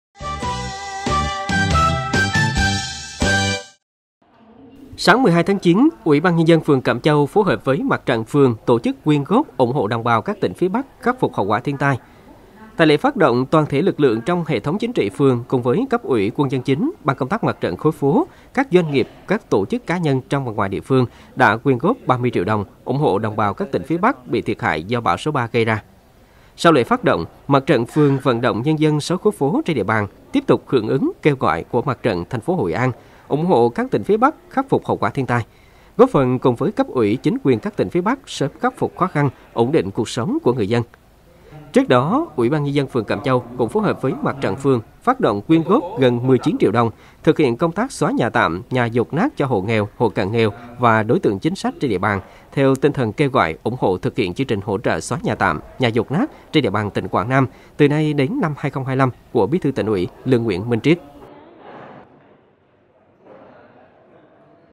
Cẩm Châu quyên góp 30 triệu đồng ủng hộ đồng bào các tỉnh phía Bắc thiệt hại do bão lũ
Thực hiện: Mỹ Lệ - Minh Vũ
PTV: Phan Sơn